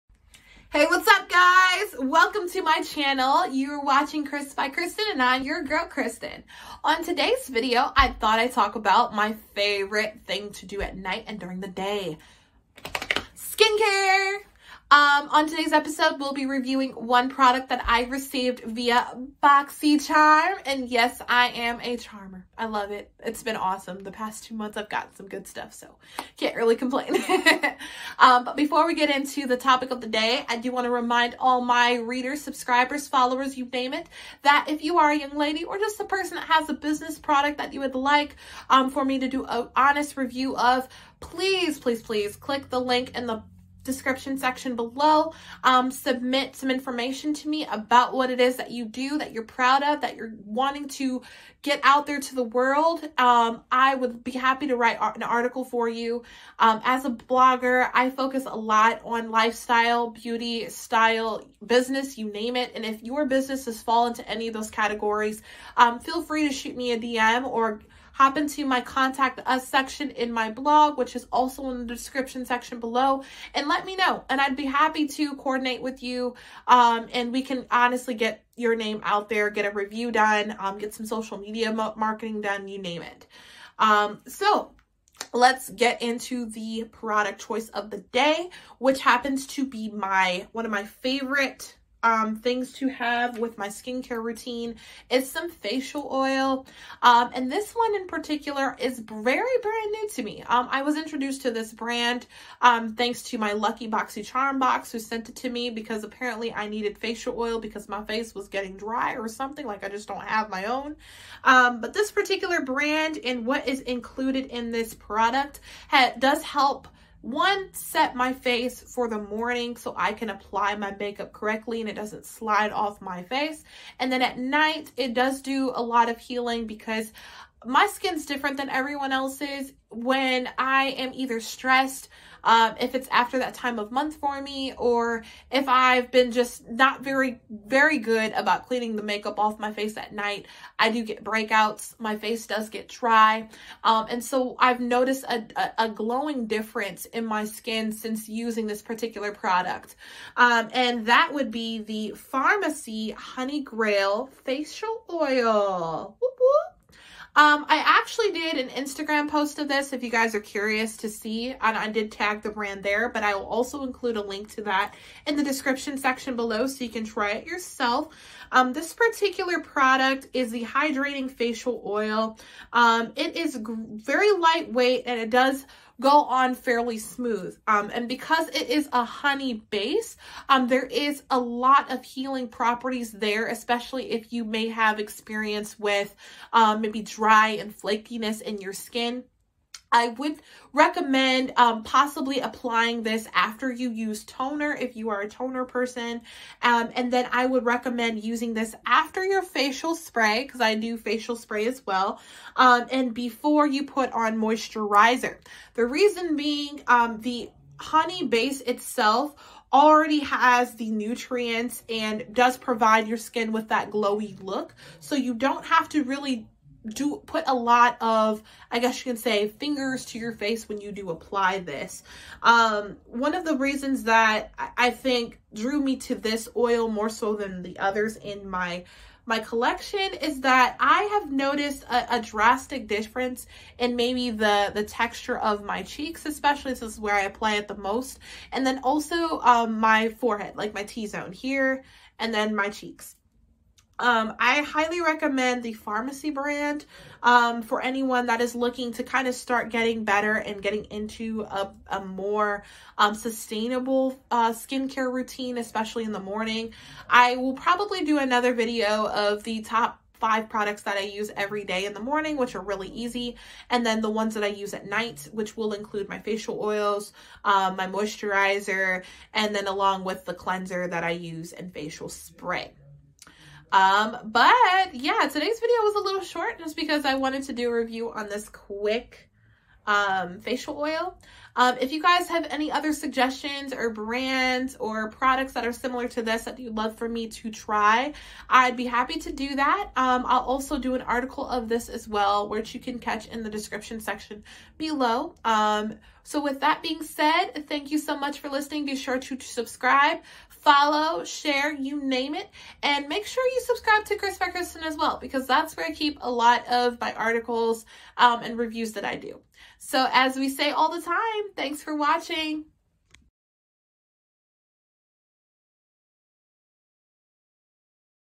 Honey Grail Facial Oil Review | Sweeten Up Your Skin Care Routine
On Chris by Christen we talk : Beauty , Style, Mom Hacks and More

Allow me to introduce you to a brand of Facial oil that has proven to give hydration, eliminate redness, and also provide my skin ( dry) that glow that I am looking for. I have also been able to give my skin a break from heavy make up like foundation and concealer when I apply this in the morning on a chill day.

Introducing Honey Grail Facial Oil By Farmacy